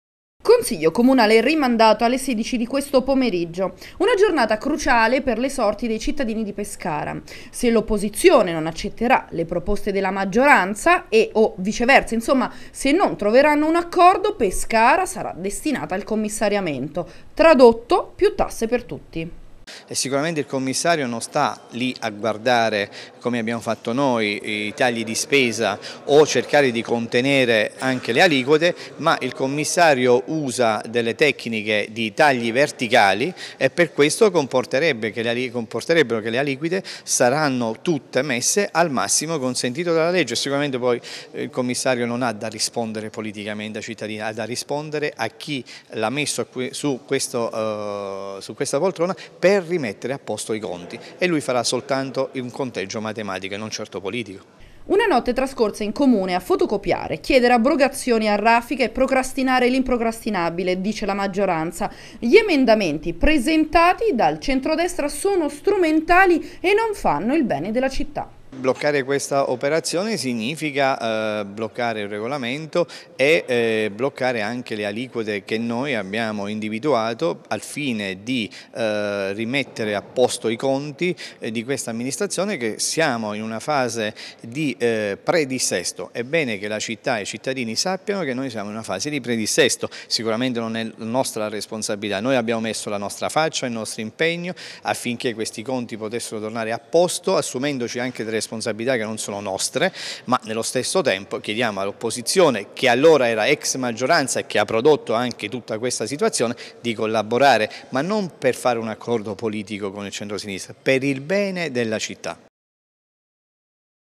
Pescara, Teodoro: "Le responsabilità non sono nostre"
Seconda giornata di consiglio comunale a Pescara, ancora lontano l'accordo tra maggioranza e opposizione, seduta sospesa fino al pomeriggio nell'ultimo giorno utile per l'approvazione della Iuc